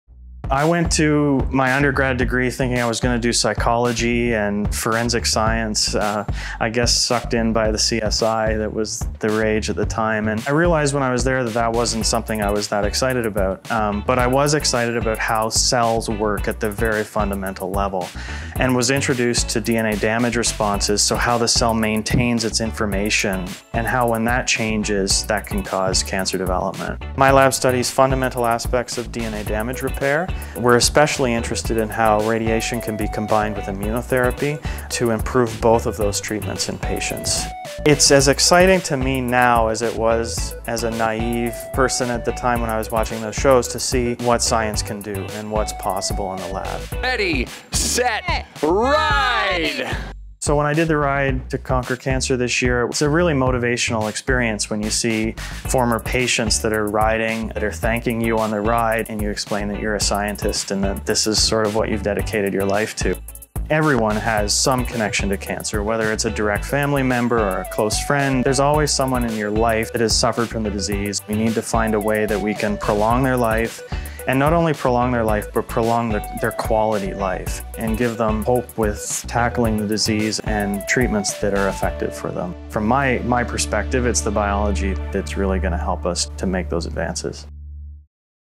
Fighting cancer with your immune system - Shane Harding, Science in the 6ix
Meet Dr. Shane Harding from the Princess Margaret Cancer Centre who is studying how combining cancer treatments may improve patient outcomes.